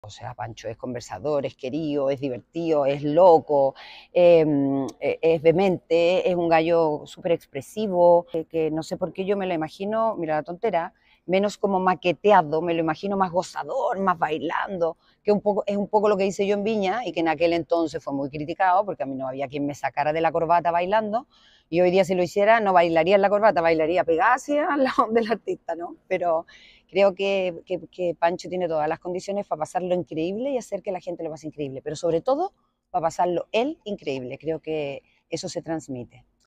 Eva Gómez sobre los atributos de Pancho Saavedra para animar Viña 2024#festivaldeviña
La exanimadora del Festival de Viña del Mar, Eva Gómez, comentó los atributos de Francisco Saavedra para ser el nuevo animador de Viña 2024 junto a María Luisa Godoy.

La comunicadora señaló que ‘Pancho’ tiene todas las cualidades para cumplir un excelente papel en la Quinta Vergara, pero también para pasarlo bien en el festival más grande de Latinoamérica.

Lee más de lo que nos contó en Pagina7.cl